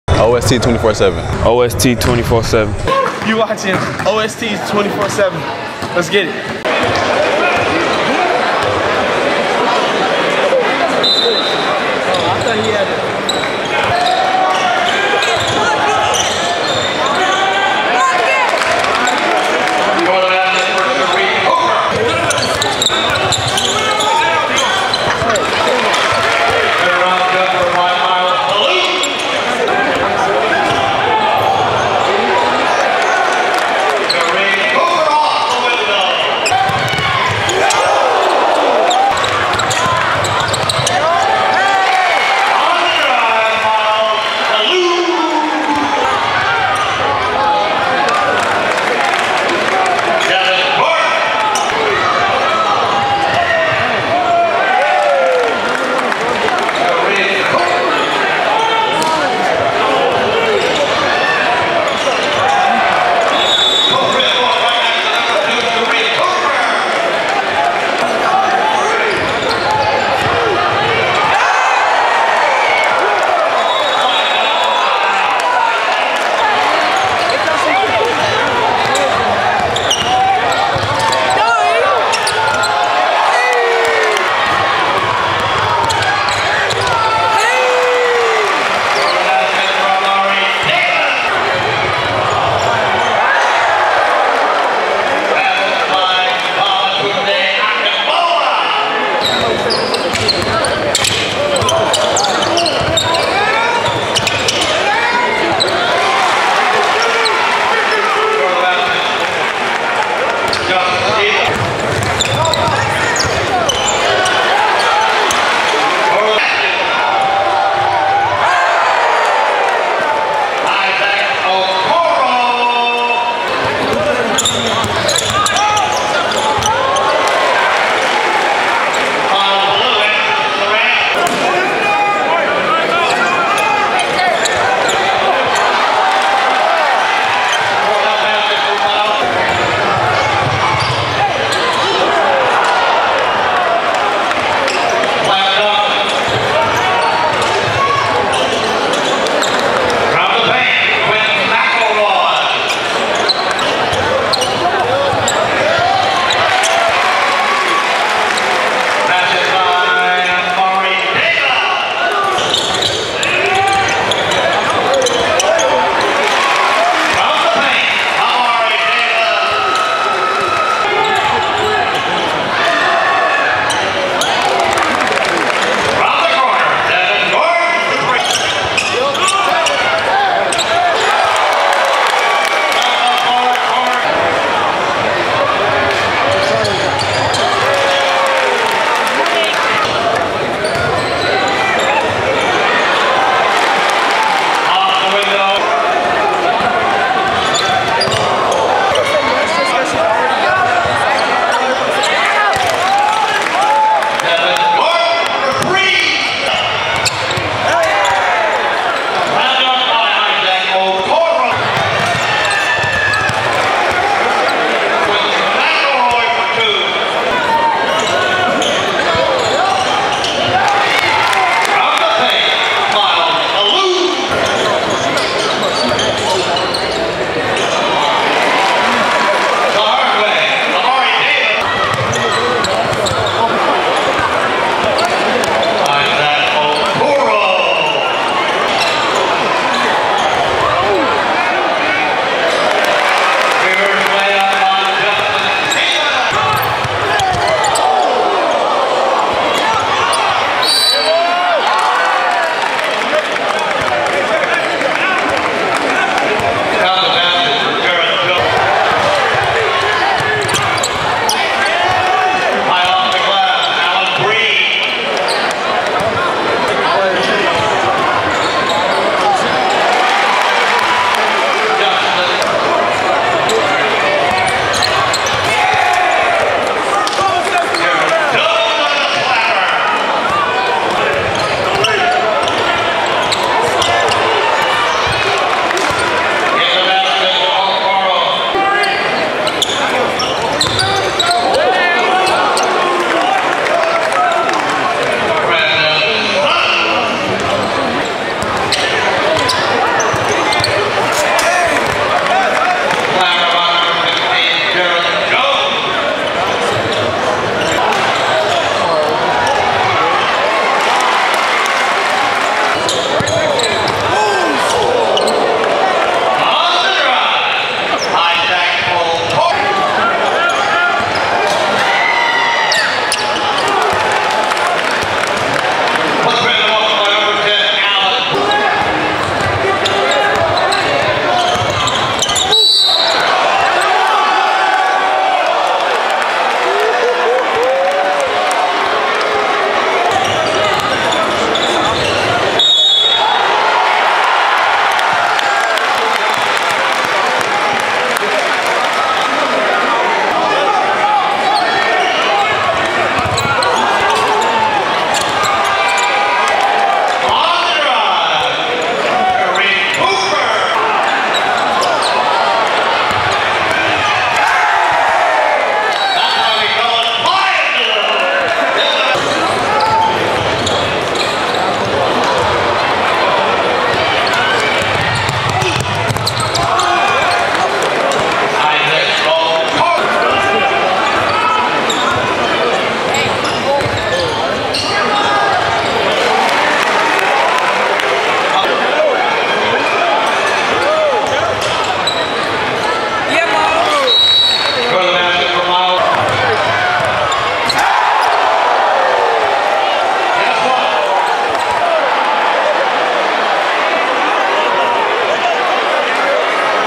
McEachern SHOWS OUT in Flying to The Hoop DEBUT | FULL HIGHTLIGHTS
McEachern took their talent on the road to Dayton Ohio. They faced a good team from OHIO Trotwood. Isaac Okoro had 25 pts. Sharife Cooper had 23 pts for McEachern. For Trotwood, Amari Davis had 24 points and Myles Belyue had 19 points

AFTER YOU FINISH, DON’T FORGET TO HIT THAT THUMBS UP BUTTON UP BUTTON

THANKS FOR ROCKING WITH US